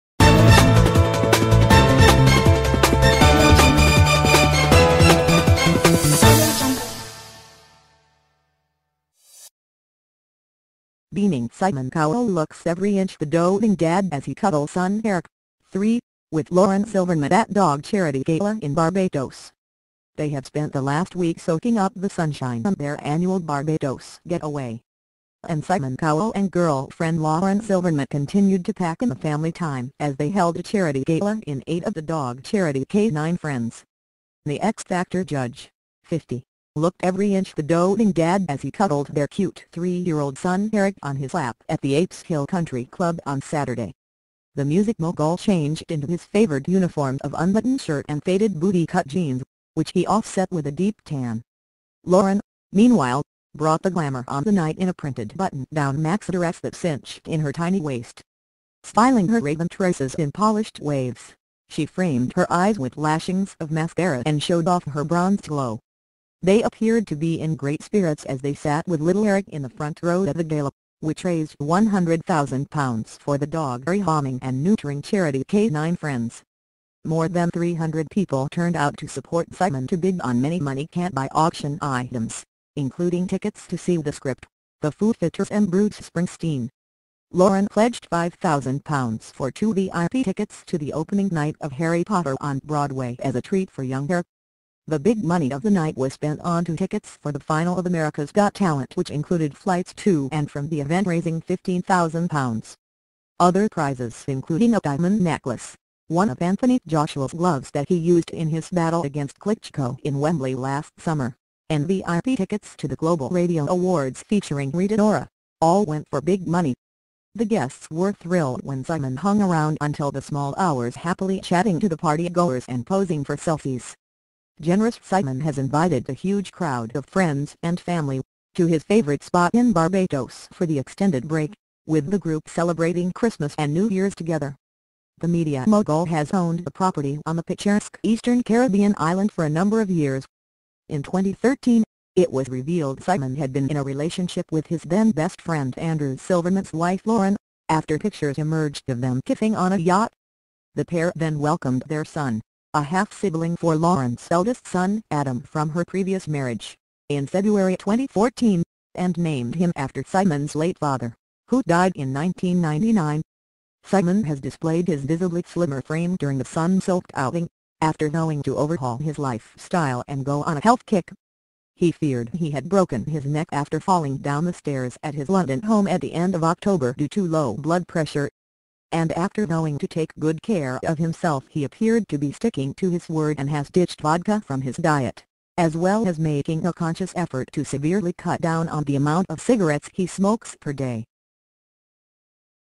Simon Cowell as he cuddles son Eric with Lauren Silverman at dog charity gala in Barbados
They have spent the last week soaking up the sunshine on their annual Barbados getaway.